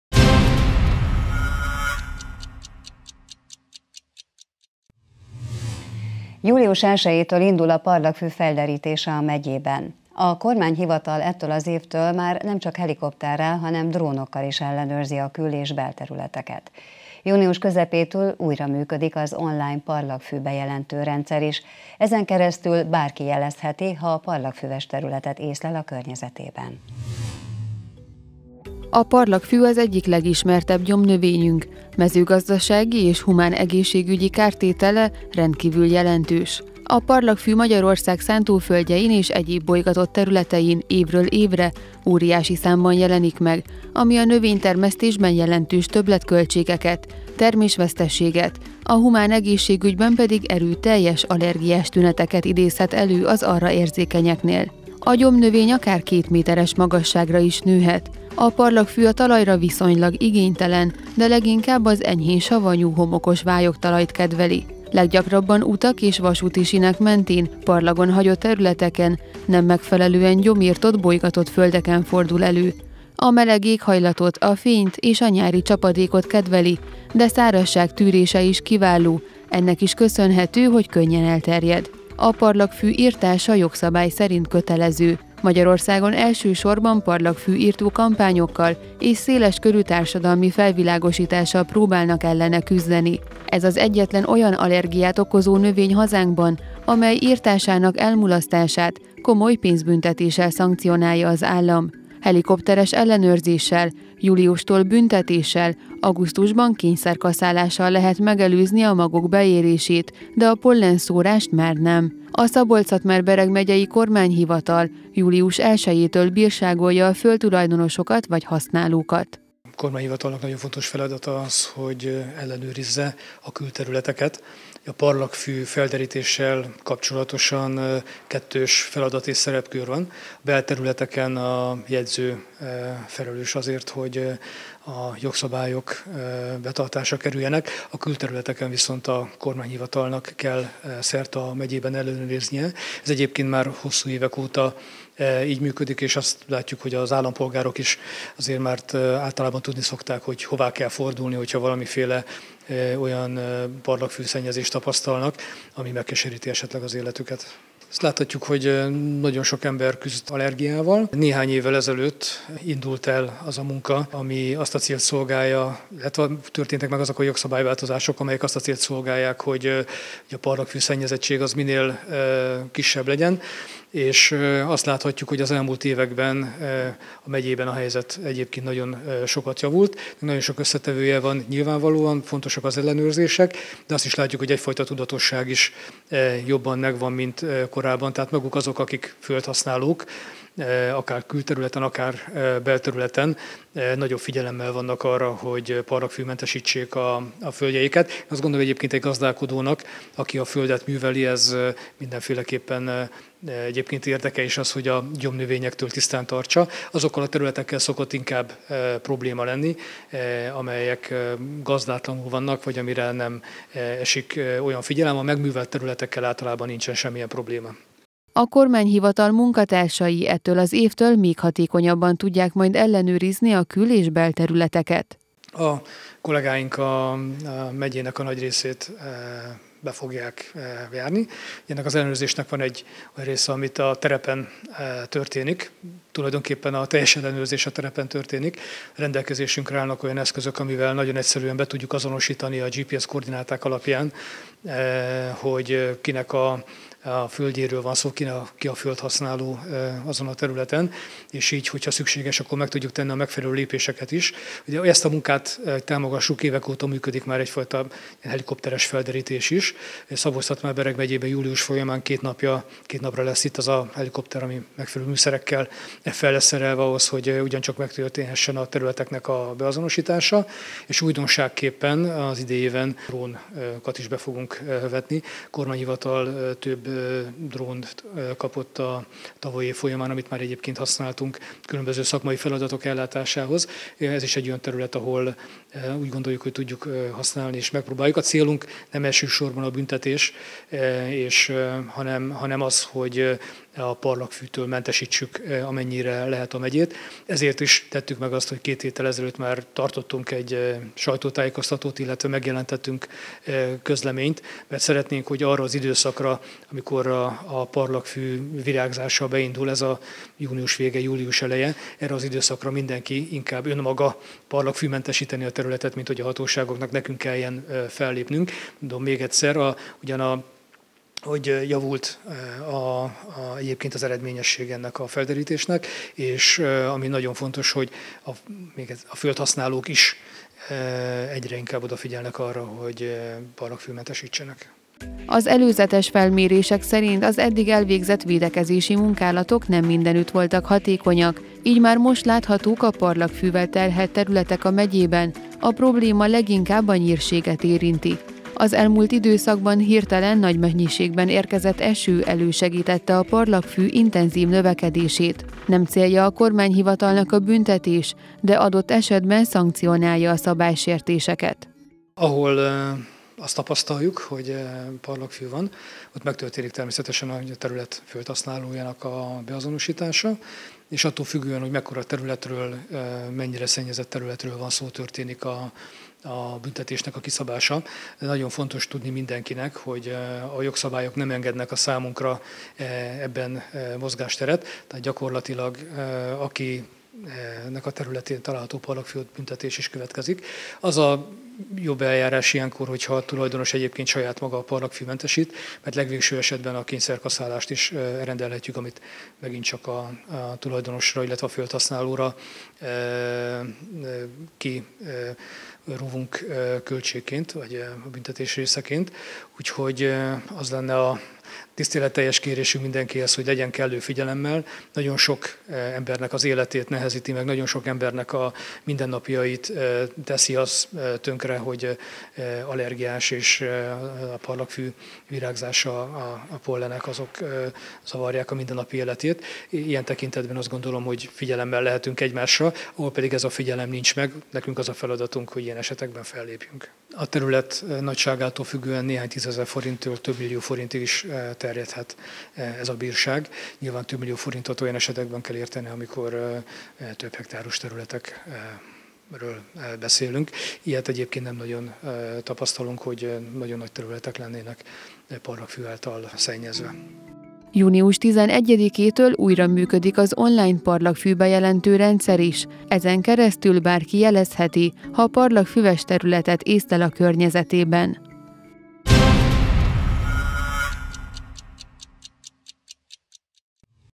35 perc/Parlagfű - Kölcsey Tv
Július elsejétől indul a parlagfű felderítése a megyében. A kormányhivatal ettől az évtől már nem csak helikopterrel, hanem drónokkal is ellenőrzi a kül- és belterüleket. Június közepétől újra működik az online Parlagfű Bejelentő Rendszer is, ezen keresztül bárki jelezheti, ha parlagfüves területet észlel a környezetében.